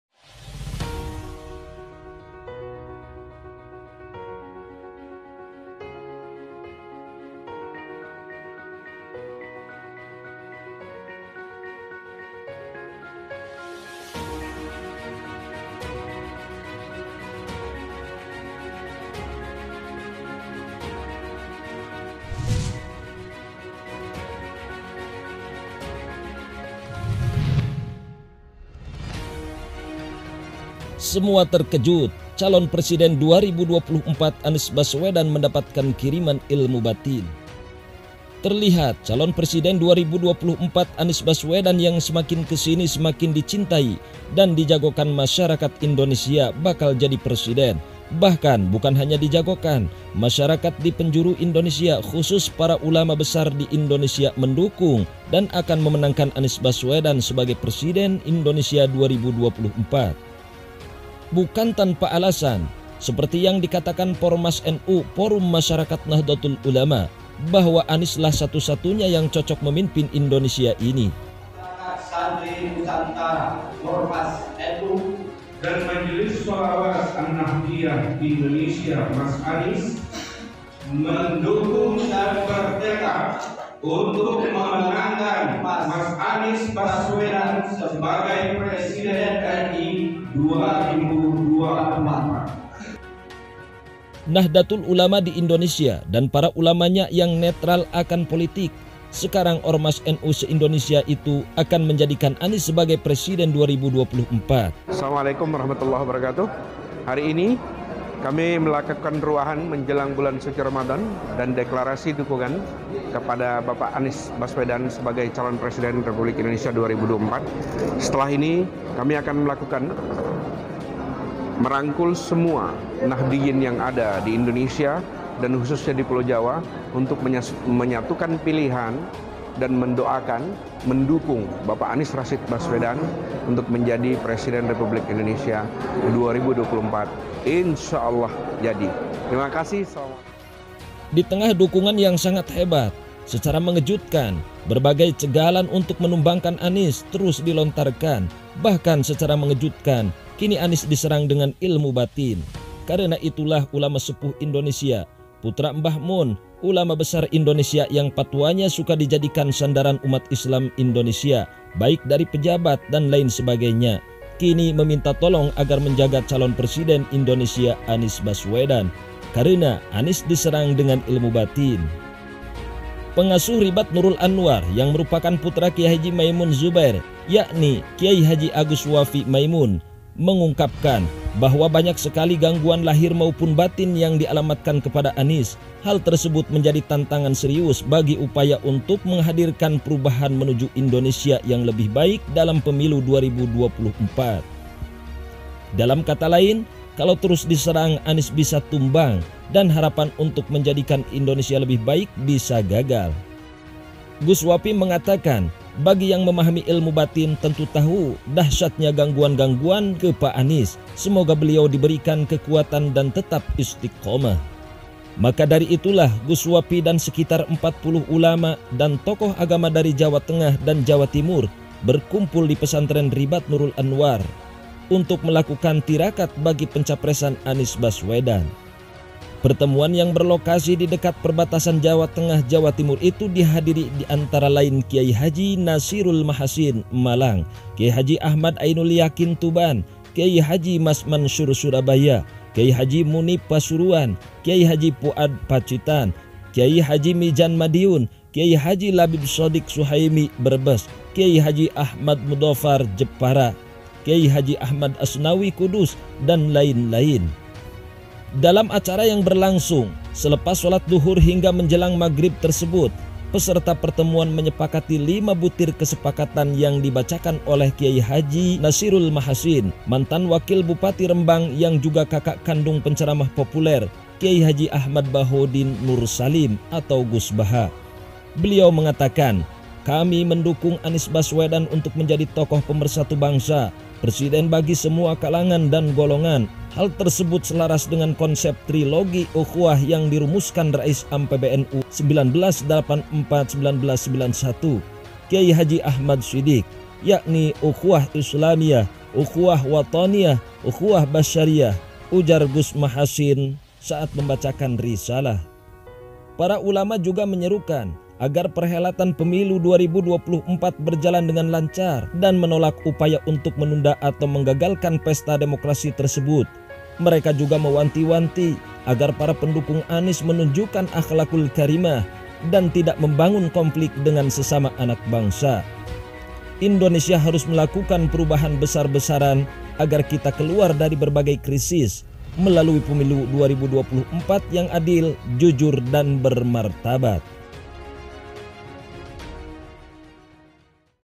KENA SANTET. 40 ULAMA LANGSUNG MERUQIAH CAPRES ANIES BASWEDAN
40 ulama tanah jawa melakukan tirakat untuk menjaga capres 2024 Anis Baswedan dari gangguan ilmu hitam. !

Selengkapnya silahkan disimak sampai selesai agar paham dengan baik dan benar

Terimakasih
Official Channel Semangat 5 Zaman